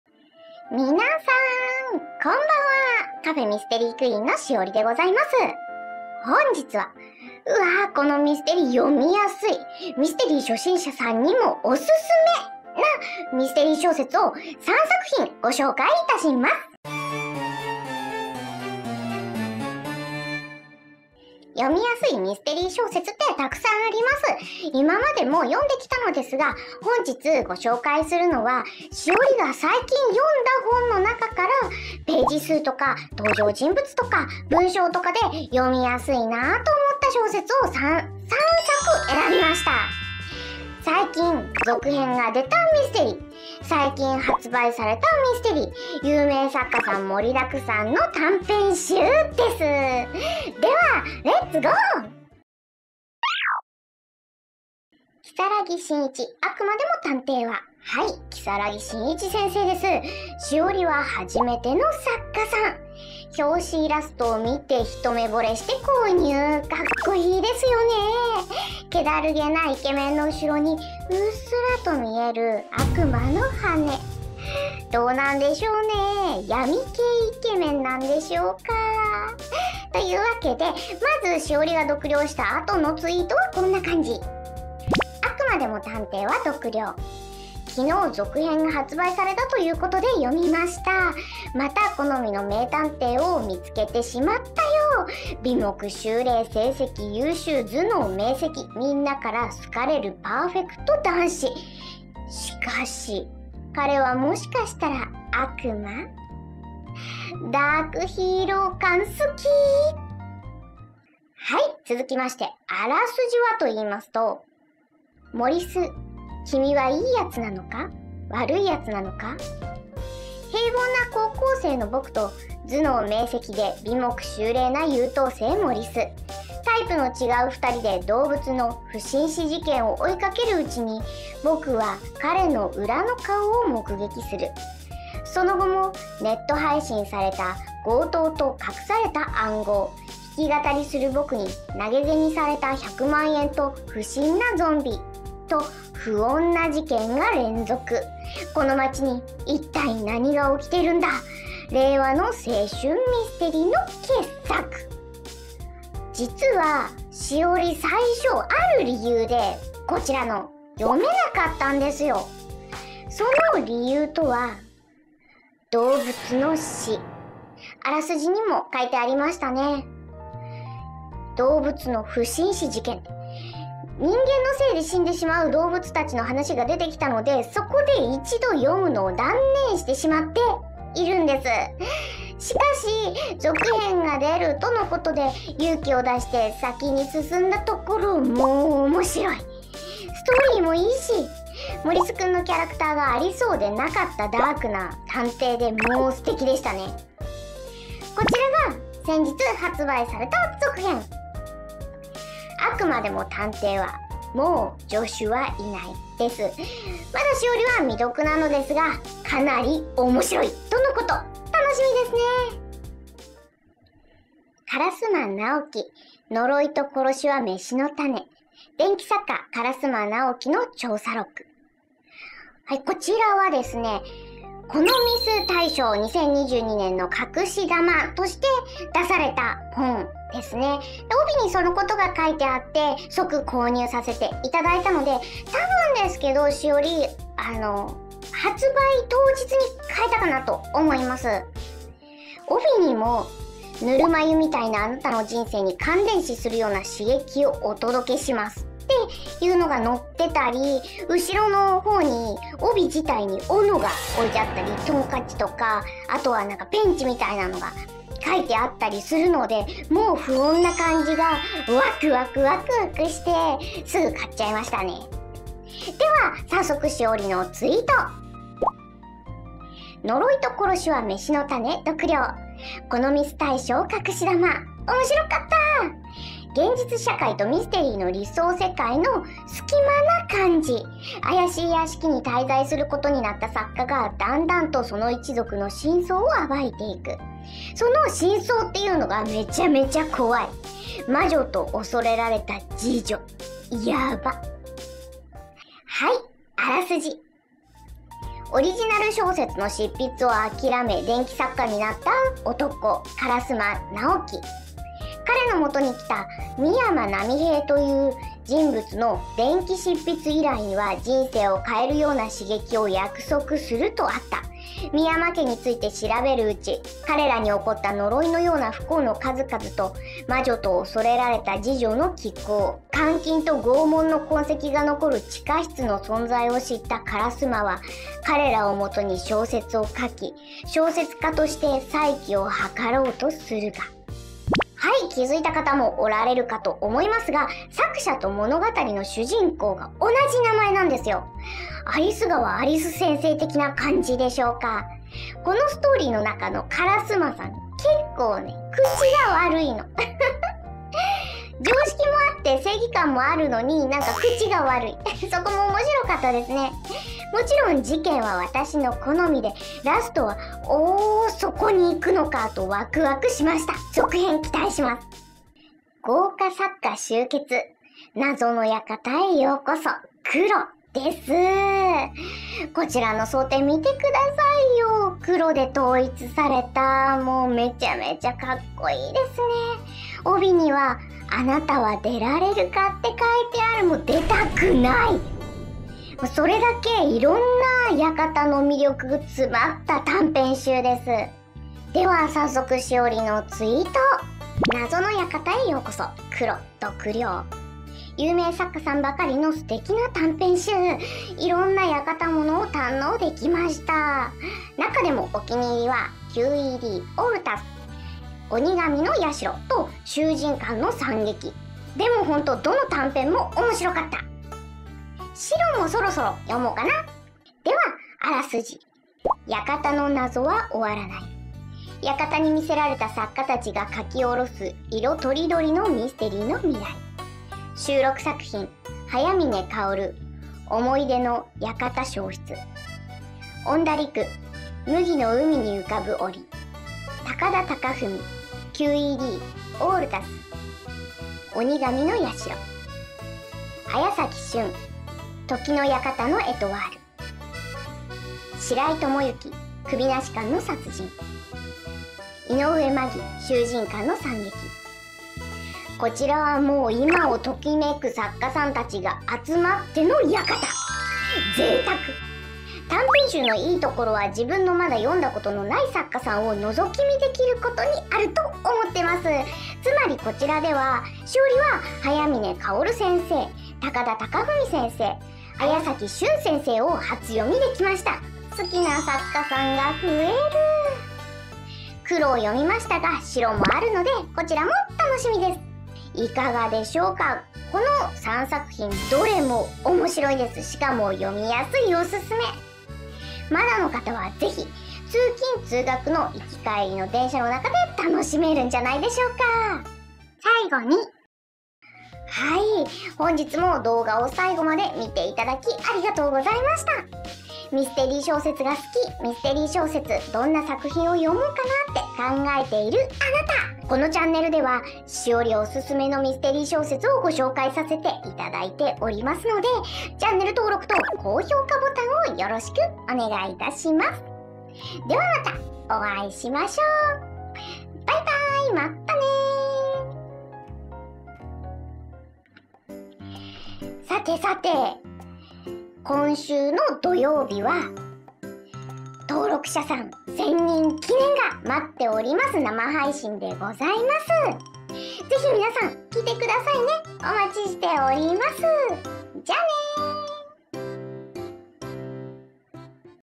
【ミステリ小説３冊紹介】「初心者さんも読みやすい！」高校生探偵・このミス隠し玉・珠玉の館短編集、オススメな小説です【複数紹介】【書評】
Bookcafe”ミステリークイーン”スタッフの栞(シオリ)です！

読みやすいミステリ小説を三冊
ご紹介しています❗️
これからの作品も楽しみな作家さんばかり✨
まだの方ぜひ✨
気になっている方の参考になれば嬉しいです☆

～～🔖目次～～
00:00 オープニング
00:55 今回のオススメ小説１冊目
03:33 ２冊目
06:06 ３冊目
08:21 さいごに
08:52 エンディング

⭐Twitterやブログもよかったら見てください(•ө•)♡

⭐３Dモデル製作
「栞」　自作です✨

⭐２Dモデル製作
「栞」　FELUCCA様
「アリス」　いしのに様

🎨【画像・イラスト提供】
●イラストAC様

♪【BGM・SE提供】
■DOVA-SYNDROME様